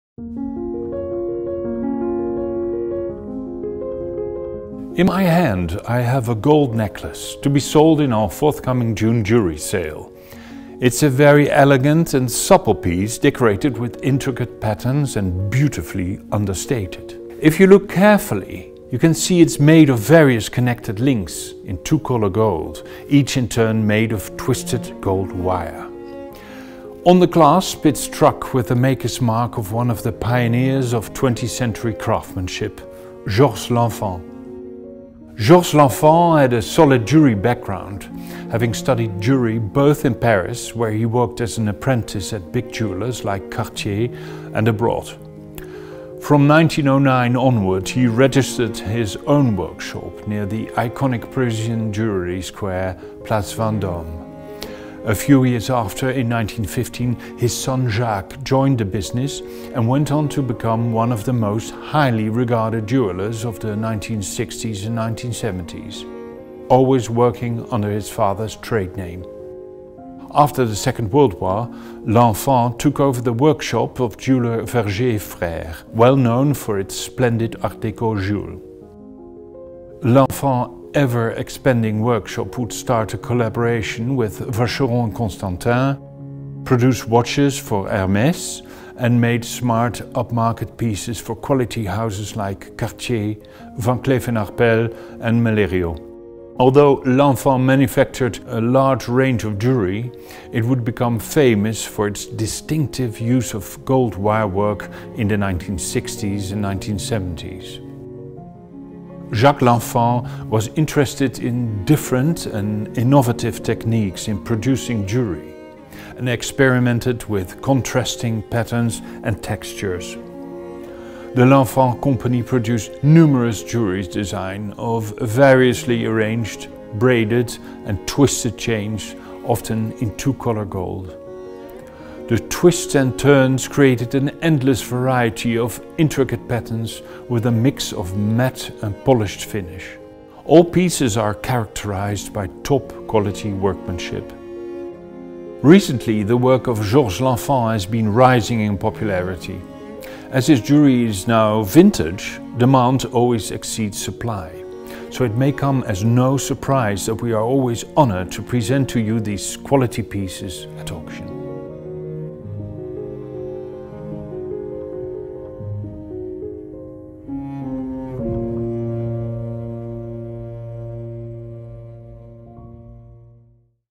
Expert's voice | A vintage and elegant two-colour gold necklace by Georges Lenfant at auction
Daniel Girod presents this elegant two-colour gold necklace by Georges Lenfant up for sale in our Exclusive Jewels auction of June 23.